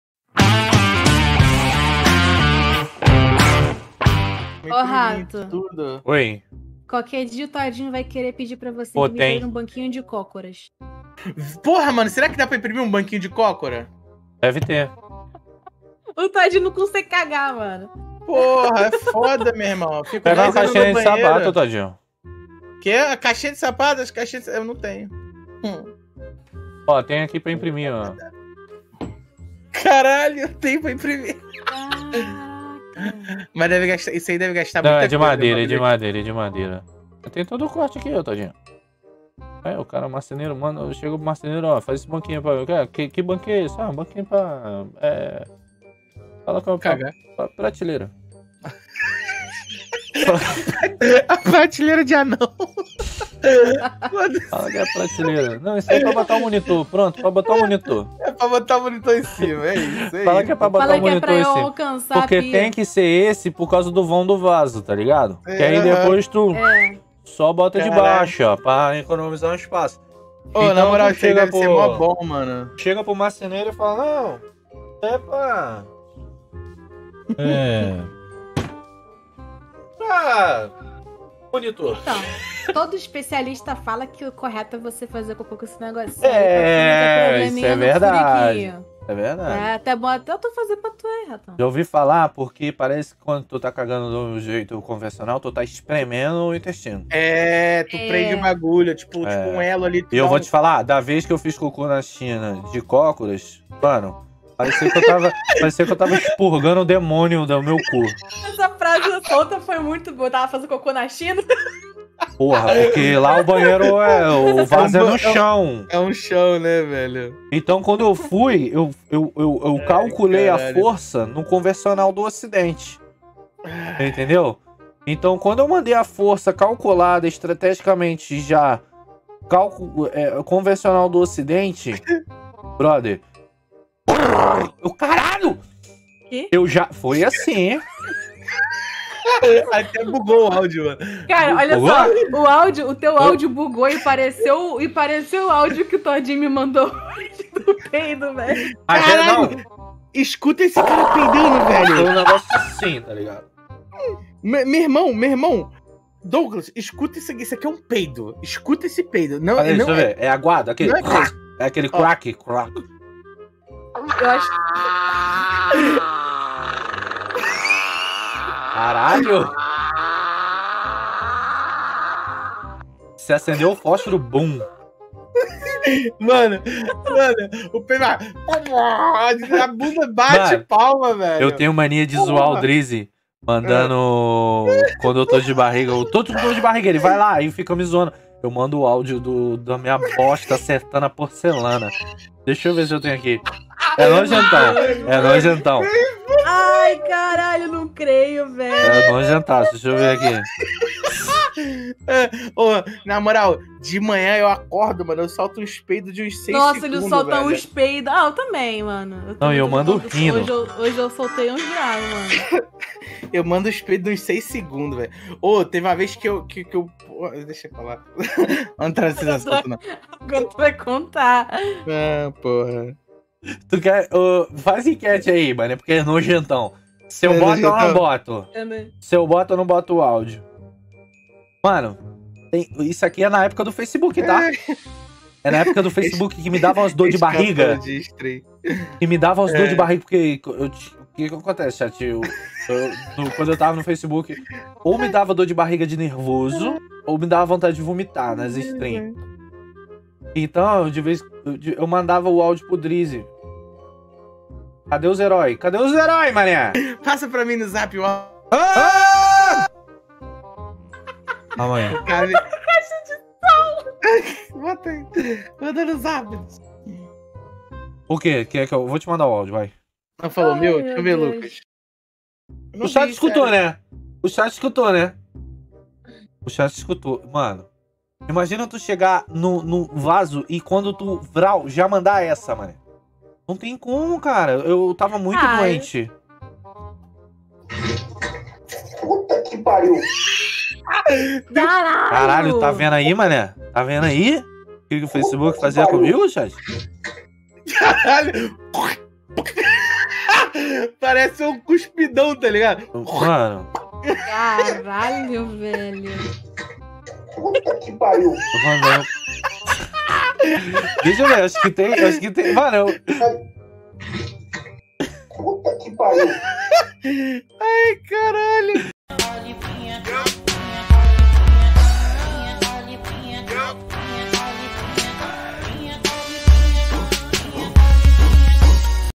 SÓ ASSISTA SE TIVER CORAGEM! 😂 Douglas, Toddynho e Pandora | Tente não rir com esse vídeo
Melhores momentos da live rato borrachudo e Jefinho.

Este é mais um corte da live do Rato Borrachudo. Acompanhe os cortes e toda a diversão das lives do Rato Borrachudo aqui no canal Cortes Borrachudos.